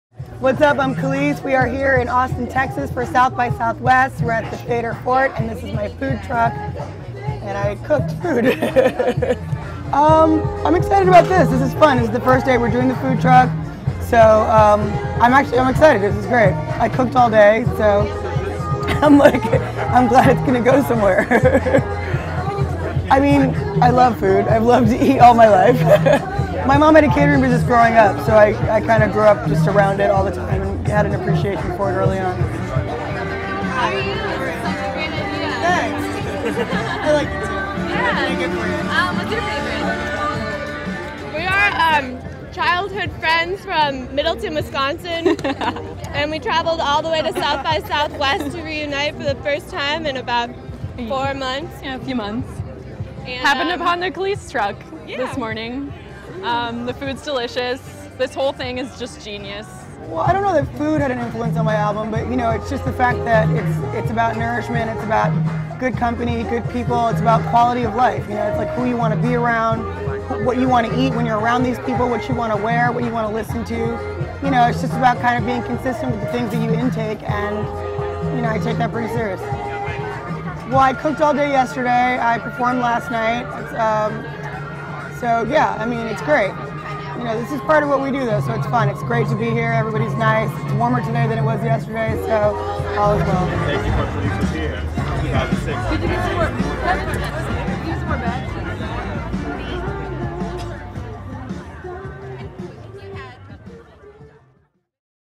Kelis Dishes Out Free Food at SXSW
On the eve of her sixth album release, R&B singer Kelis cooks and serves free food at SXSW 2014.